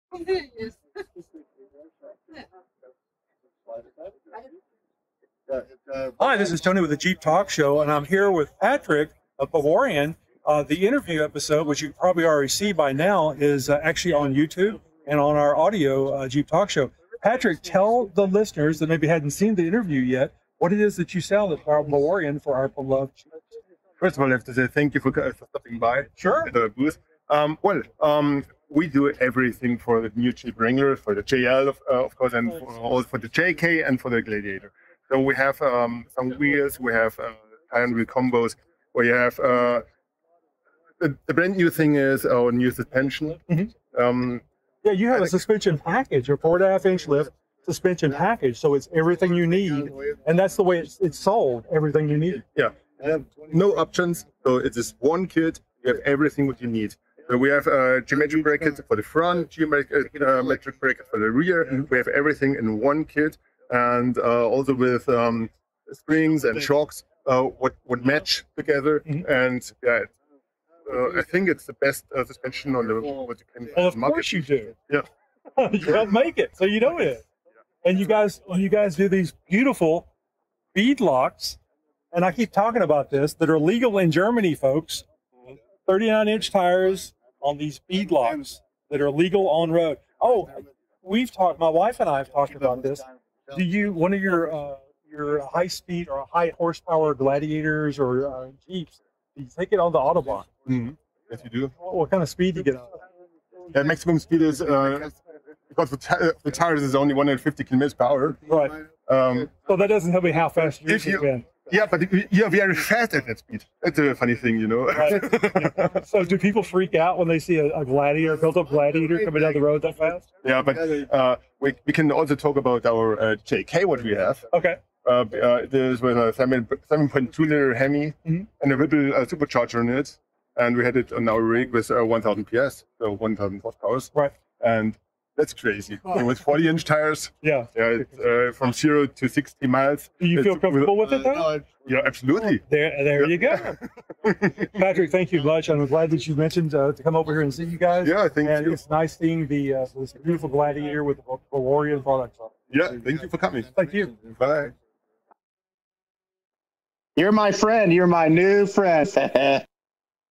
Patrick Schittko of Bawarrion.com
Tony from the Jeep Talk Show stops by the Bawarrion booth at SEMA 2024 for an exciting interview with CEO Patrick Schittko. Discover the incredible craftsmanship of Bawarrion's Gladiator, showcasing premium German engineering now available in the U.S. market. The video doesn’t do justice to the beauty, strength, and precision of these Jeep products. Could you imagine driving a HEMI-powered, lifted Jeep on the Autobahn?